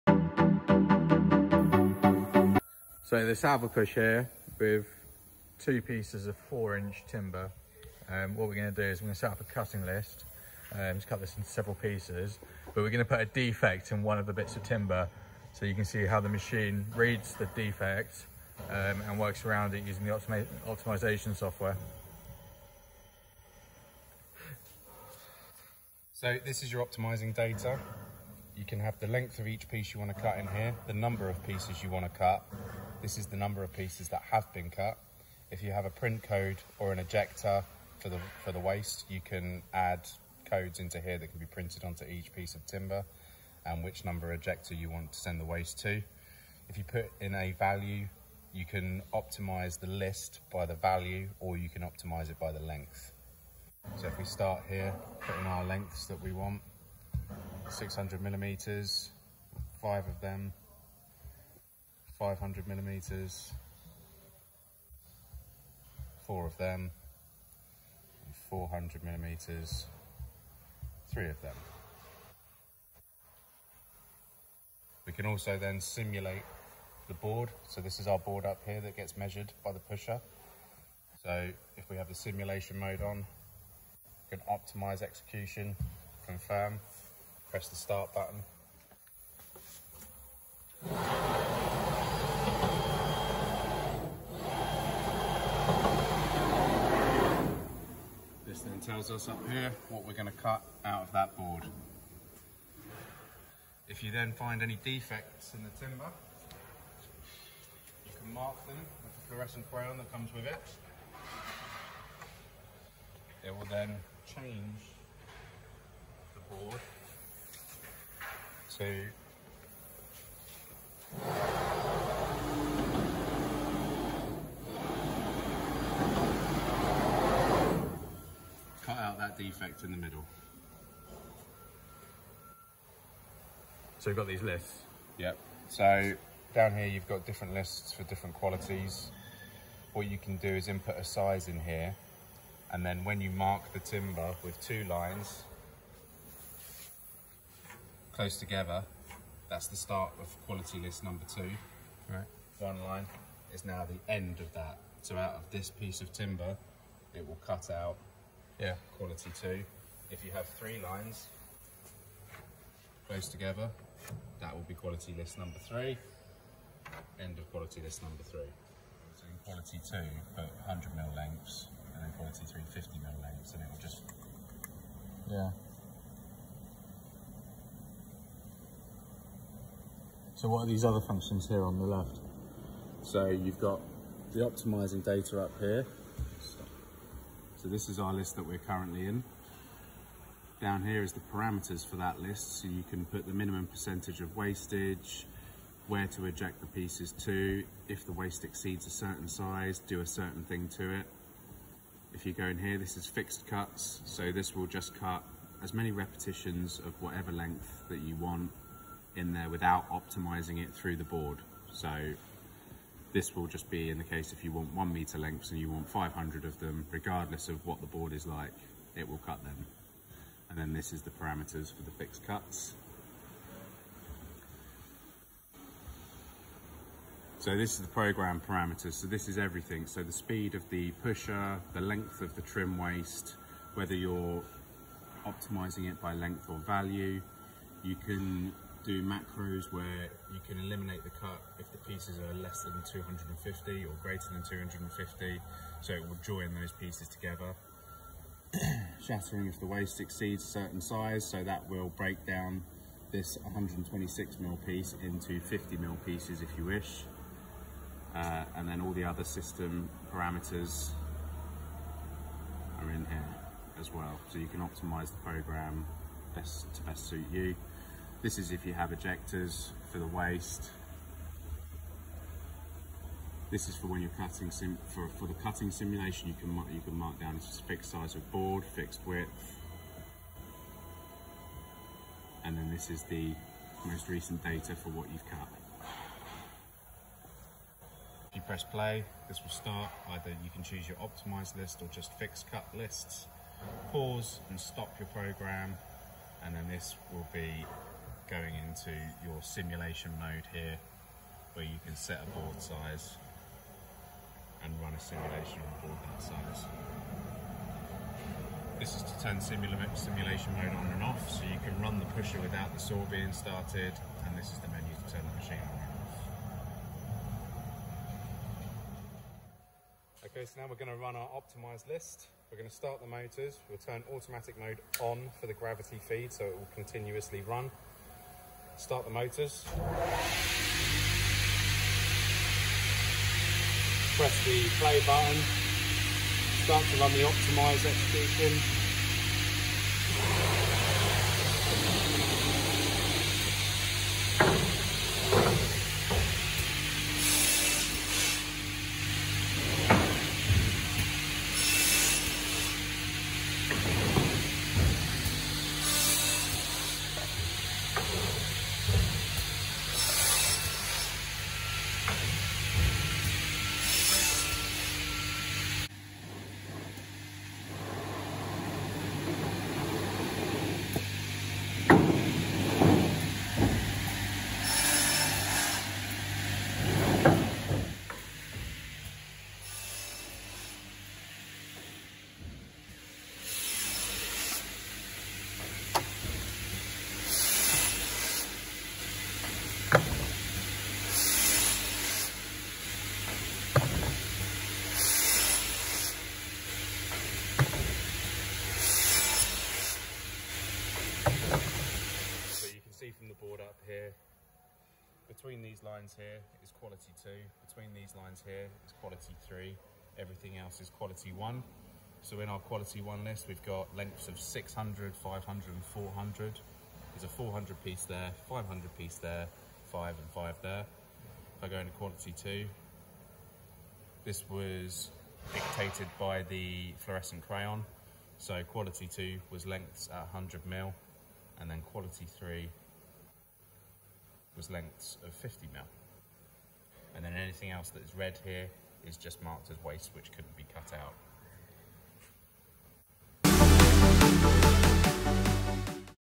SALVAMAC OPTIMISING SAW SALVAPUSH 2000 DEMO IN SHOWROOM
A DEMO IN SHOWROOM IN UK (missing some devices of the machines) to understand in easy way how the optimizing saw works